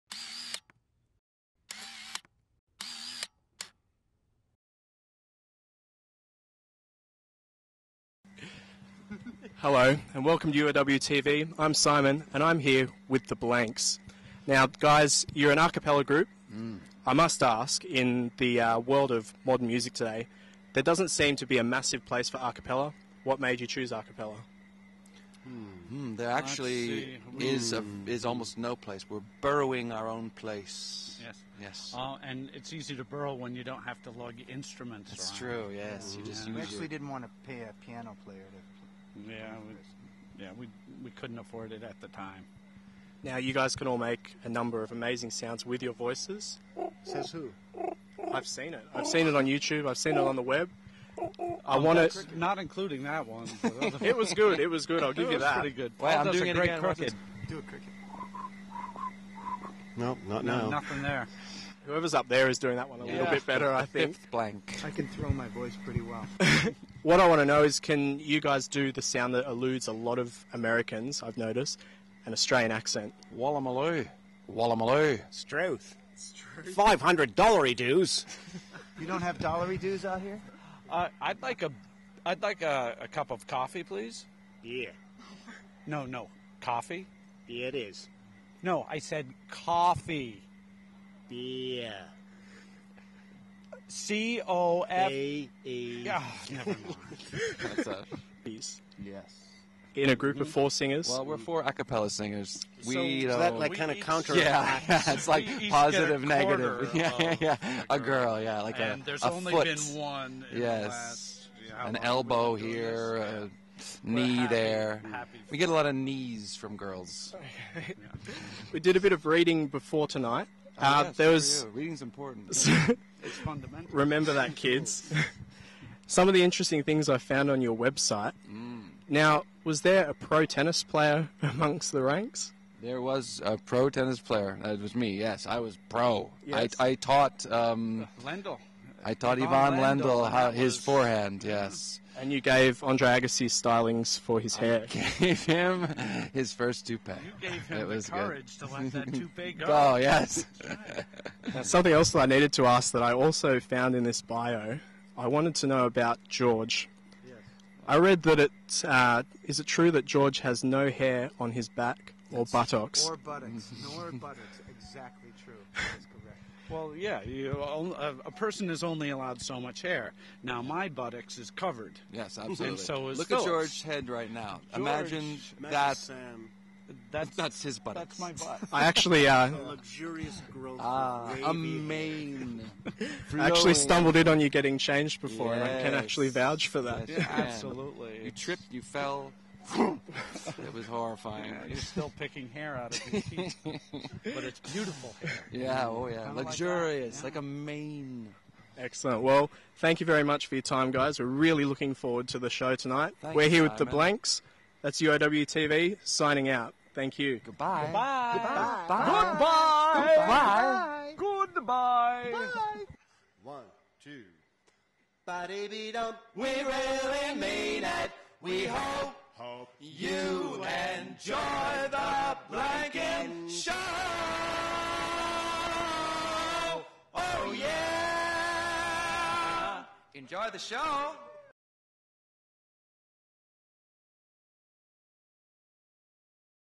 THE BLANKS AUSTRALIAN TOUR UOWTV INTERVIEW (EXTENDED CUT)
We caught up with the Blanks aka Ted's Band from Scrubs on their recent Australian Tour